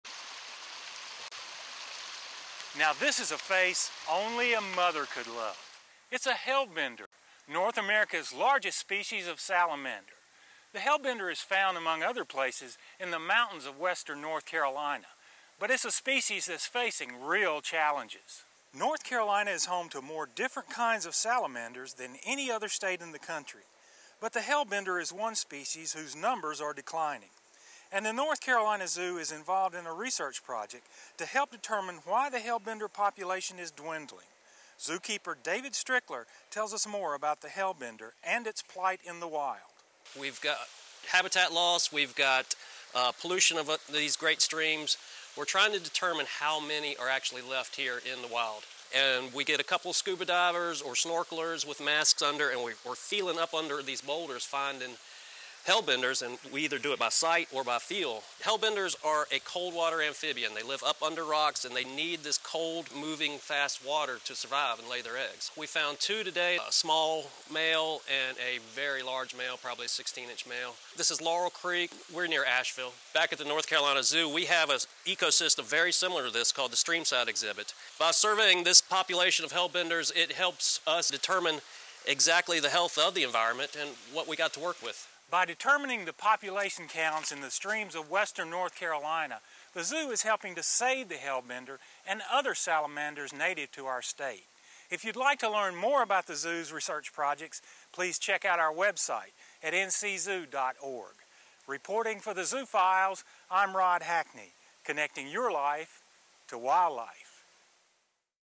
Hellbender Research in North Carolina
The hellbender has a face only a mother could love! It is North America's largest salamander, and is found in the mountains of North Carolina. Hellbender populations face several challenges, including dwindling numbers, habitat loss, and stream pollution. A research project led by the North Carolina Zoo is working to determine how many hellbenders remain in the state and to understand the quality of the cold, fast-running streams it depends upon for habitat.